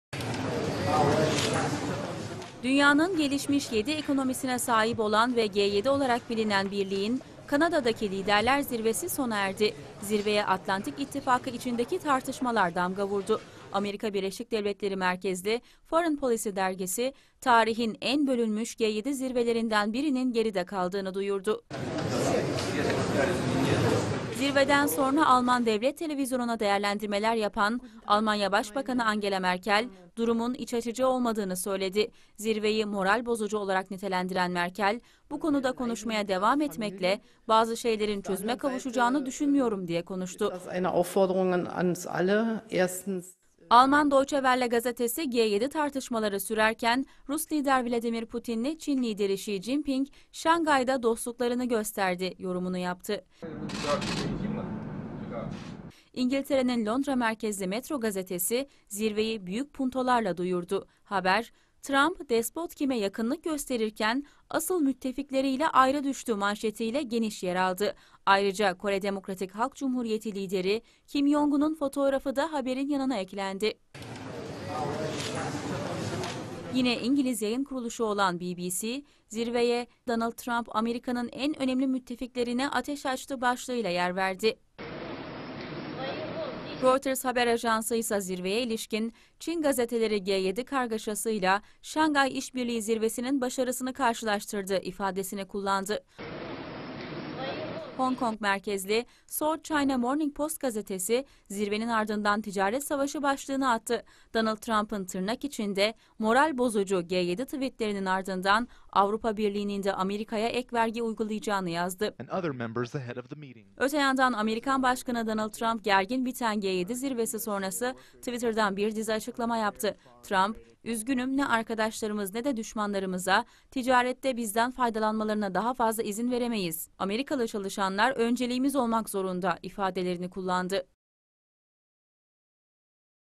Atlantik ittifakında büyük çatlak
Gergin biten G7 zirvesi sonrası, ABD Başkanı Donald Trump'ın ortak bildirgeden çekilmesi, Avrupa'da hayal kırıklığı yarattı.